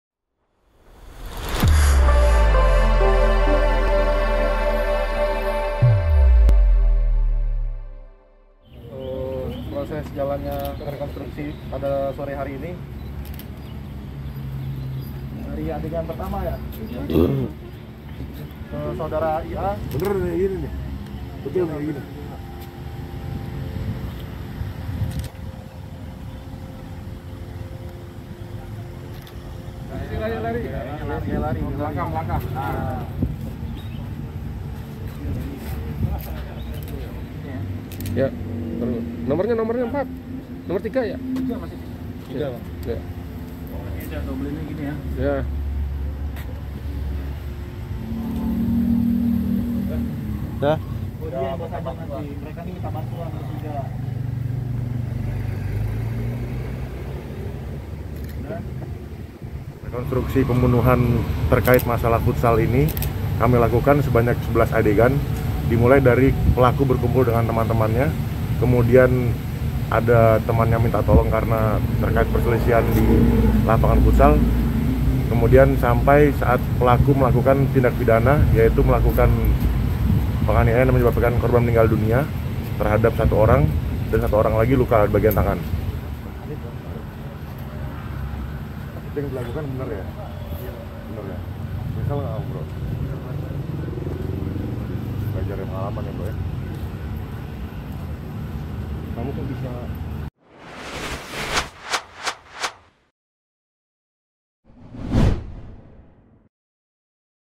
REKONSTRUKSI KASUS PEMBUNUHAN, KARENA PERMAINAN FUTSAL
JAKARTA, POSKOTA.CO.ID - Polisi gelar rekonstruksi kasus pembunuhan yang terjadi karena permainan futsal pada Senin 19 April lalu di Jalan Bulak Teko, Kalideres, Jakarta Barat.

Rekonstruksi digelar di Halaman Mapolres Metro Jakarta Barat, Selasa (25/5/2021). Ada 11 adegan yang diperagakan oleh polisi.

Dalam rekonstruksi itu, tersangka IA (23) dihadirkan langsung untuk mengetahui bagaimana proses pembacokan berujung kematian kepada korbannya yakni MMR (19).

Kanit Krimum Polres Metro Jakarta Barat, AKP Dimitri Mahendra mengatakan rekonstruksi digelar dengan memperagakan 11 adegan.

"Dimulai dari pelaku berkumpul dengan teman-temannya, kemudian ada temannya minta tolong karena terkait perselisihan di lapangan futsal," ujarnya di Polres Metro Jakarta Barat, Selasa (25/5/2021).

Kemudian adegan dilanjut dengan memperlihatkan bagaimana tersangka akhirnya bisa membacok korban pada bagian punggung hingga menyebabkan korban inisial IA tewas dan korban lain inisial P luka pada bagian tangan akibat sabetan celurit.

Hingga adegan terakhir yakni pelaku kabur ke tempat persembunyiannya.

"Pada adegan 8 dan 9 pelaku melakukan pembacokan kepada korban," ujar Dimitri.

Tidak ada barang bukti baru dari rekonstruksi yang digelar itu. Saat ini pihak kepolisian sedang mempersiapkan berkas untuk selanjutnya dituangkan ke Jaksa Penuntut Umum (JPU).

"Sedang kami tuangkan semua ke dalam berkas perkara untuk melengkapi berkas perkara ke JPU," jelasnya.

Dimitri menambahkan, tersangka nekat melakukan hal tersebut karena dalam pengaruh alkohol. Sebanyak dua botol minuman ia tenggak bersama teman-temannyam

"Dari hasil penyidikan kami pelaku mengonsumsi minuman keras sebelum melakukan tindak pidana sebanyak dua botol," tandasnya.

Sebelumnya diberitakan, seorang anak muda inisial MMR (19) tewas usai dibacok oleh tersangka yakni IA (23) di Jalan Bulak Teko, Kalideres, Jakarta Barat.

Diketahui, peristiwa ini terjadi pada Senin (19/4/2021) lalu ketika kelompok korban dan pelaku bermain futsal. Saat keduanya bertanding, kelompok korban berhasil mengalahkan kelompok pelaku.

Sebelumnya, kedua pihak sudah sepakat soal pembayaran uang sewa futsal Rp 365 ribu. Kelompok korban meminta kelompok pelaku membayar uang sewa lapangan futsal secara penuh.

Namun permintaan itu ditolak kelompok pelaku yang merasa sebelumnya kedua pihak telah sepakat untuk membayar uang sewa futsal itu secara bersama-sama. Kejadian ini kemudian menimbulkan percekcokan.

Korban yang sempat pergi menggunakan sepeda motornya rupanya dikejar oleh pelaku. Pelaku kemudian menusuk korban dengan senjata tajam.

Korban sempat dilarikan ke rumah sakit. Namun nyawa korban tidak tertolong. (CR01).